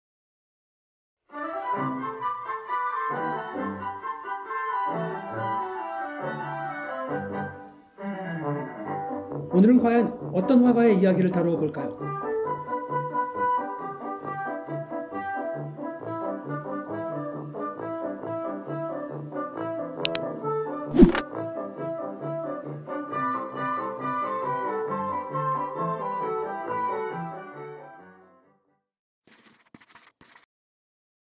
(르네 마그리트 티져) 초현실주의 대가 르네 마그리트의 이야기와 특별전 스포일러
르네 마그리트 영상 티져 입니다.